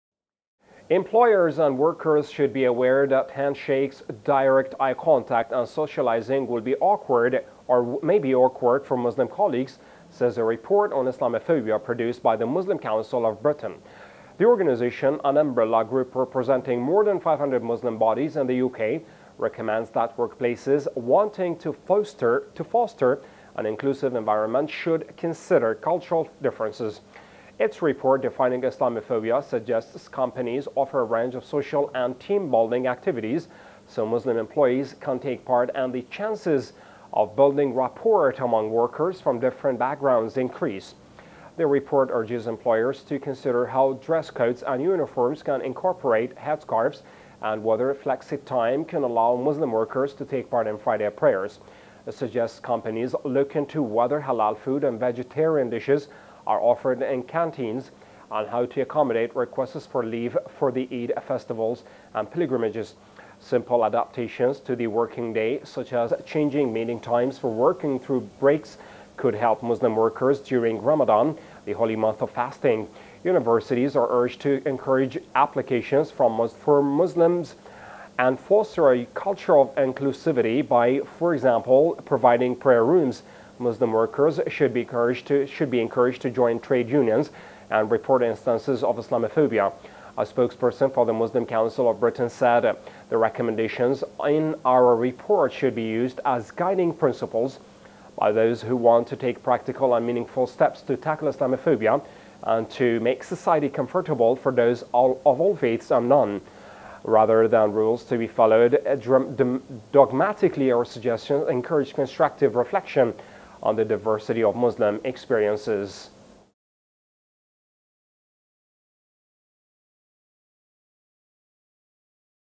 Muslim Council of Britain: Workplace habits may make Muslim colleagues uncomfortable
Employers and workers should be aware that handshakes, direct eye contact and socializing may be awkward for Muslim colleagues, says a report on Islamophobia produced by the Muslim Council of Britain.
The organization, an umbrella group representing more than 500 Muslim bodies in the UK, recommends that workplaces wanting to foster an inclusive environment should consider cultural differences.
Its report, Defining Islamophobia, suggests companies offer a range of social and team-building activities so Muslim employees can take part and the chances of building rapport among workers from different backgrounds increase.
The report urges employers to consider how dress codes and uniforms can incorporate headscarves, and whether flexitime can allow Muslim workers to take part in Friday prayers.
It suggests companies look into whether halal food and vegetarian dishes are offered in canteens, and how to accommodate requests for leave for the Eid festivals and pilgrimages. Simple adaptations to the working day, such as changing meeting times or working through breaks, could help Muslim workers during Ramadan, the holy month of fasting.
Universities are urged to encourage applications from Muslims and foster a culture of inclusivity by, for example, providing prayer rooms.
Muslim workers should be encouraged to join trade unions and report instances of Islamophobia.
A spokesperson for the Muslim Council of Britain said: “The recommendations in our report should be used as guiding principles by those who want to take practical and meaningful steps to tackle Islamophobia and to make society comfortable for those of all faiths and none. Rather than rules to be followed dogmatically, our suggestions encourage constructive reflection on the diversity of Muslim experiences.”